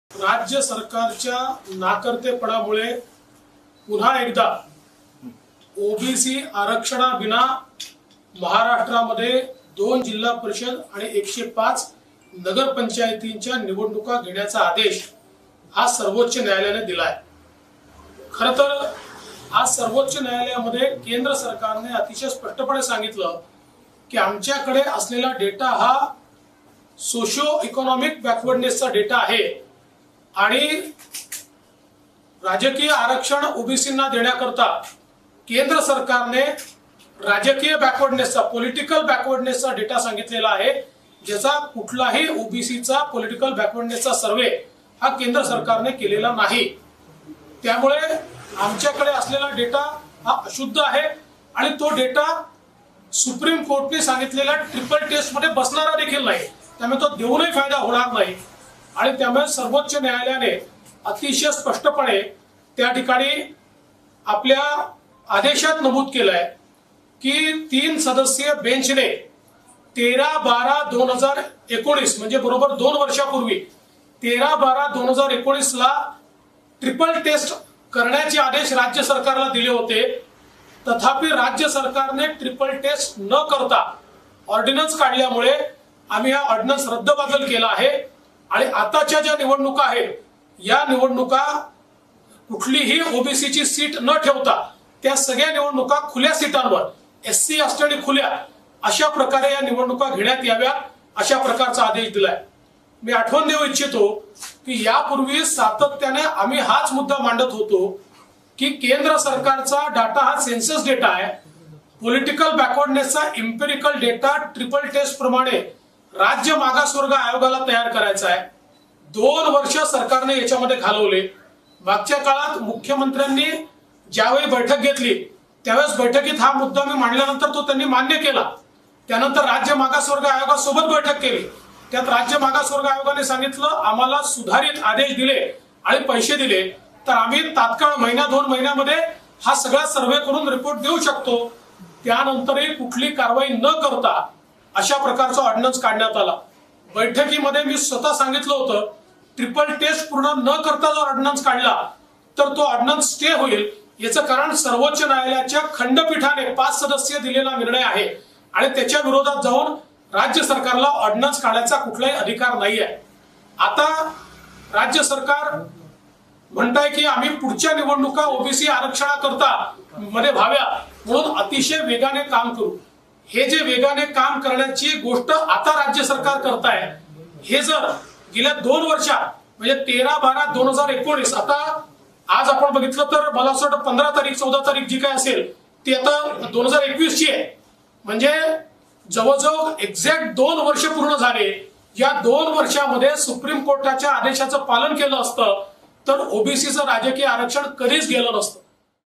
OBC Reservation | फडणवीसांची महाविकास आघाडी सरकारवर सडकून टीका | Sakal Media
OBC Political Reservation | फडणवीसांची महाविकास आघाडी सरकारवर सडकून टीका
राज्य सरकारच्या ओबीसींसाठीच्या सर्व मागण्या सर्वोच्च न्यायालयाने फेटाळून लावल्या आहेत. यावर प्रतिक्रिया देताना राज्याचे विरोधी पक्षनेते देवेंद्र फडणवीस (Devendra Fadnavis) यांनी महाविकास आघाडी सरकारवर सडकून टीका केली. राज्य सरकारच्या नाकर्तेपणामुळे पुन्हा एकदा ओबीसी आरक्षणाविना निवडणूका घेण्याचे आदेश आहेत. खरंतर केंद्र सरकारने आजही पुन्हा स्पष्ट केले के आमच्याकडे असलेले डाटा हा केवळ सामाजिक-आर्थिक दृष्ट्या मागास वर्गाचा डाटा आहे, आणि ओबीसींना राजकीय आरक्षण देण्याकरता न्यायालयाला राजकीय दृष्ट्या मागास सिद्ध करणारा डाटा हवा आहे. केंद्र सरकारने असा कोणताही सर्वे केलेला नाही. त्यामुळे आपल्याकडे असलेले डाटा हा अशुद्ध आहे आणि तो डेटा 'ट्रिपल टेस्ट' मध्ये बसणारा नाही. असेही केंद्राने न्यायालयात स्पष्ट केले. आम्ही देखील आतापर्यंत हेच सांगत होतो.